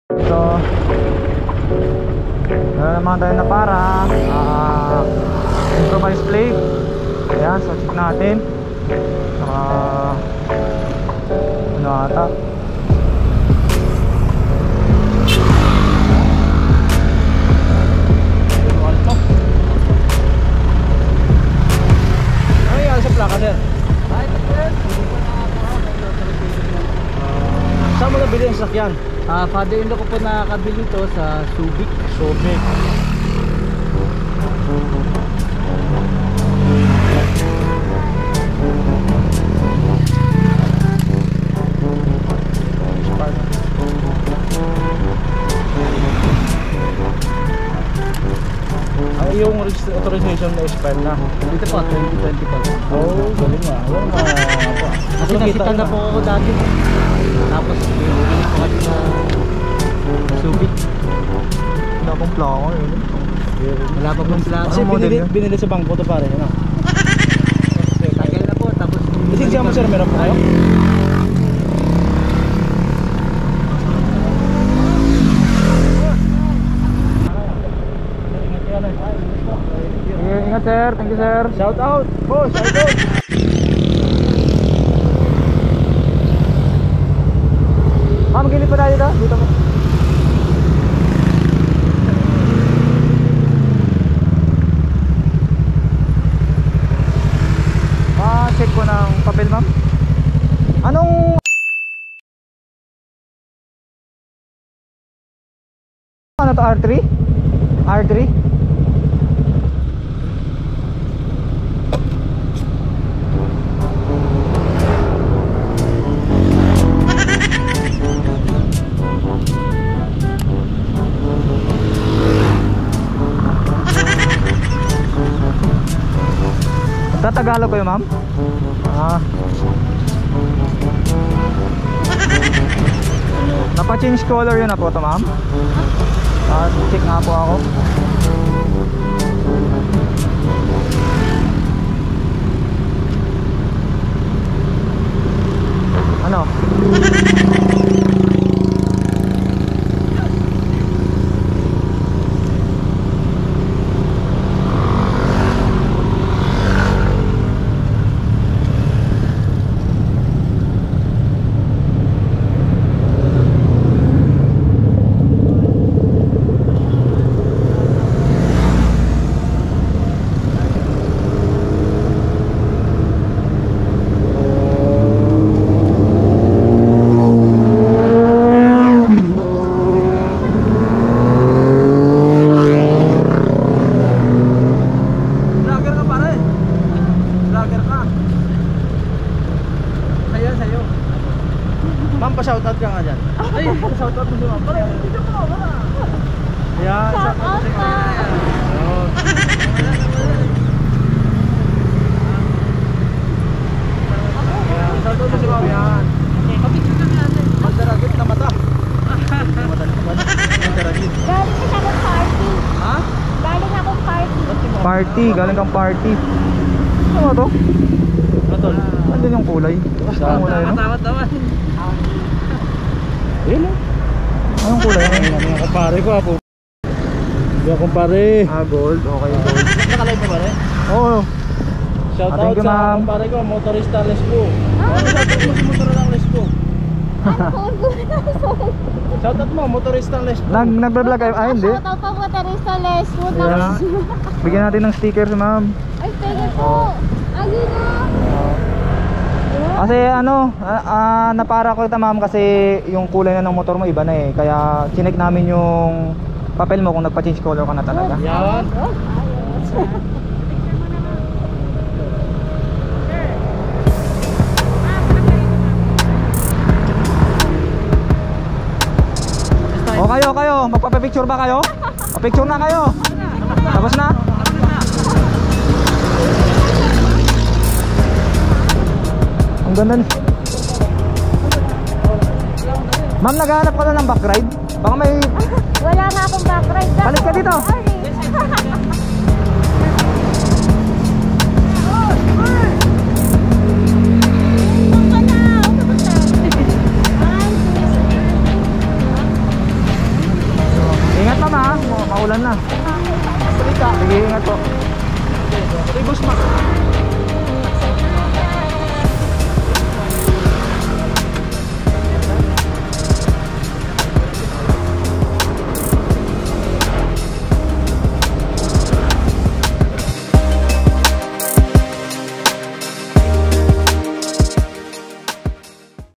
AKALA KO MA NOSEBLEED AKO SA LADY RIDER NA NASITA NATIN
Kagandahan minsan ng nag checheckpoint akala mo seryoso pero kwela pala ibang mga rider :D
Ride safe always mga buddy